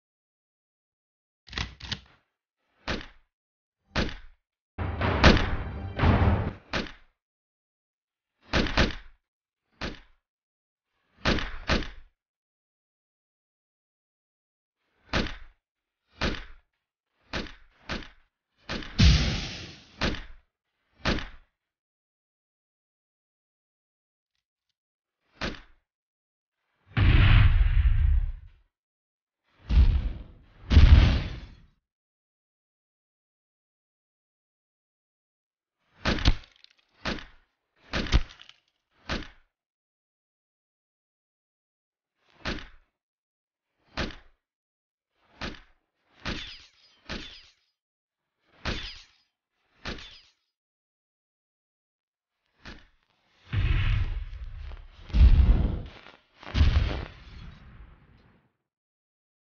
Visual Effects Shots with Action Essentials 2
Some test shots with Action Essentials 2. Features muzzle flashes and explosions. Featuring Greg Lancaster and shot on location at Cockatoo Island.